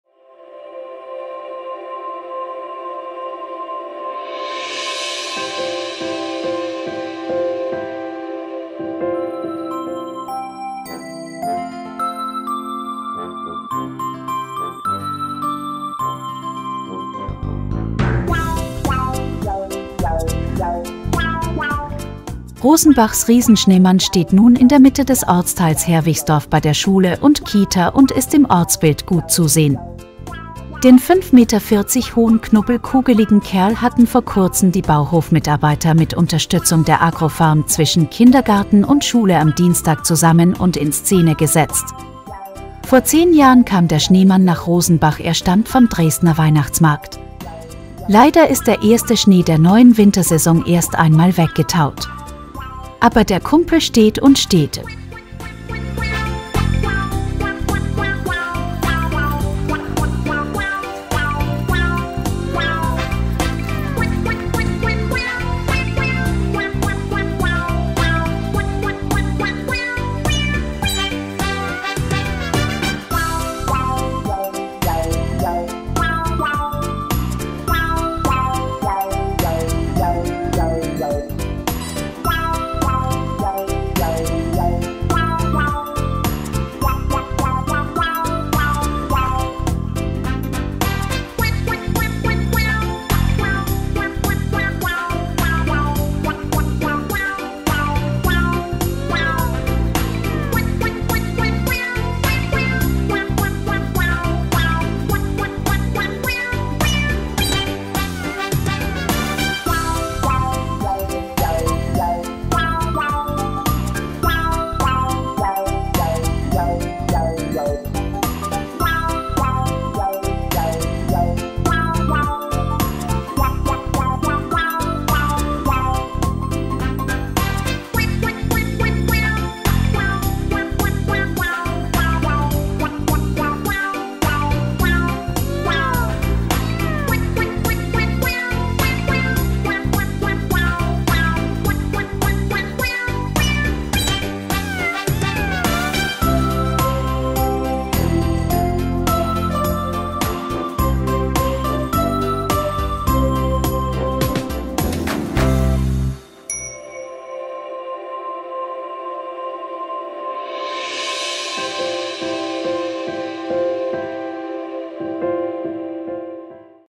Riesenschneemann in Herwigsdorf
Er stammt vom Dresdner Weihnachtsmarkt: Rosenbachs Riesenschneemann
Die Figur steht jetzt bei Schule und Kita und ist im Ortsbild gut zu sehen. Vor zehn Jahren kam der Schneemann nach Rosenbach -

Eimi  Handyclip